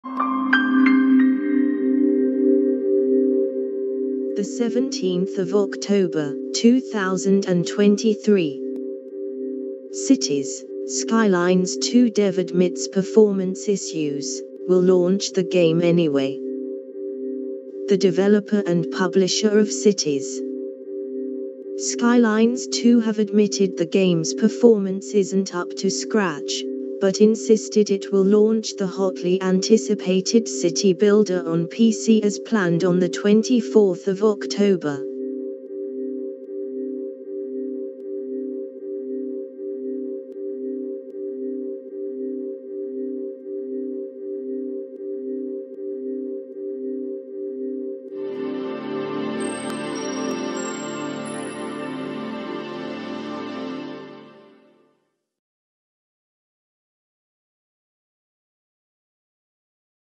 [Latest]Cities: Skylines 2 Dev Admits Performance Issues, Will Launch the Game Anyway
[Contents]
The developer and publisher of Cities: Skylines 2 have admitted the game’s performance isn't up to scratch, but insisted it will launch the hotly anticipated city builder on PC as planned on October 24.